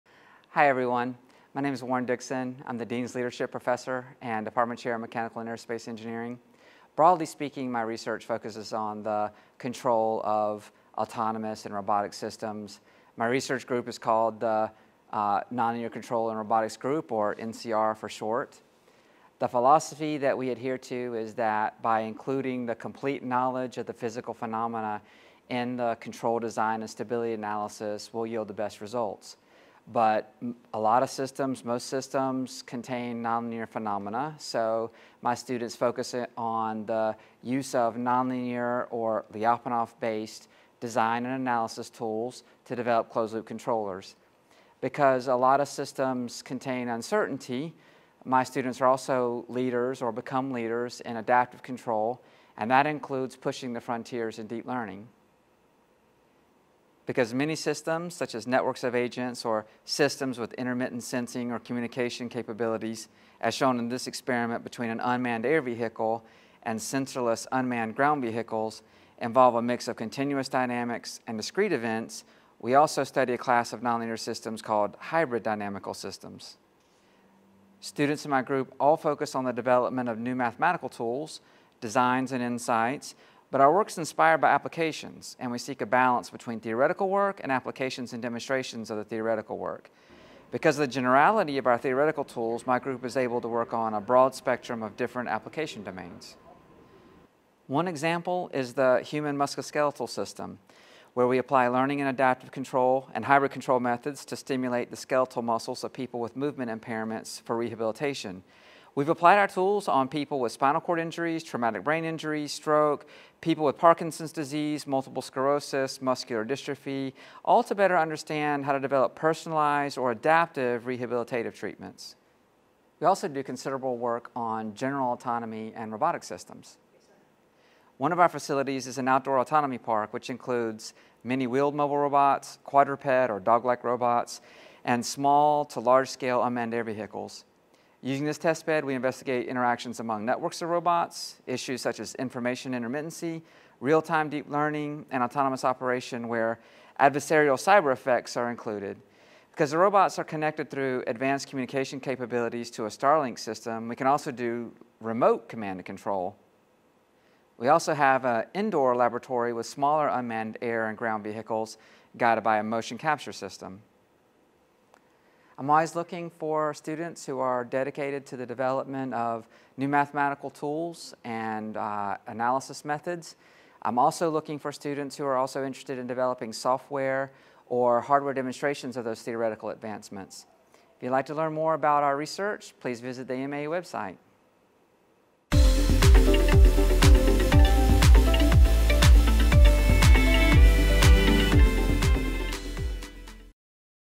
Nonlinear Controls and Robotics Group - Warren Dixon
Dr. Dixon provides an overview of the research conducted by the Nonlinear Controls and Robotics group.